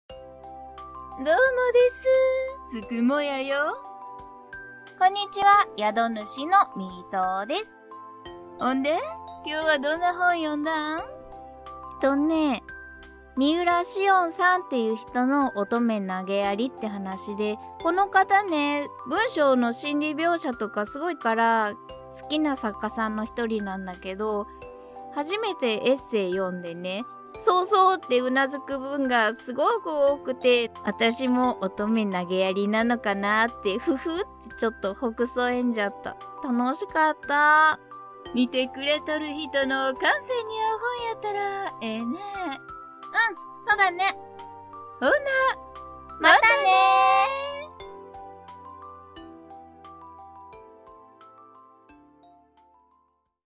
【宿主の読書感想】乙女なげやり
内容（「BOOK」データベースより）
ひとはいつまで乙女を自称しても許されるものなのか。そんな疑問を胸に抱きつつも、「なげやり」にふさわしいのは、やっぱり乙女。熱愛する漫画の世界に耽溺し、ツボをはずさぬ映画を観ては、気の合う友と妄想世界を語り合う。気の合わない母との確執も弟とのバトルも、日常の愉楽。どんな悩みも爽快に忘れられる「人生相談」も収録して、威勢よく脱力できる、痛快ヘタレ日常エッセイ。

Amazonリンク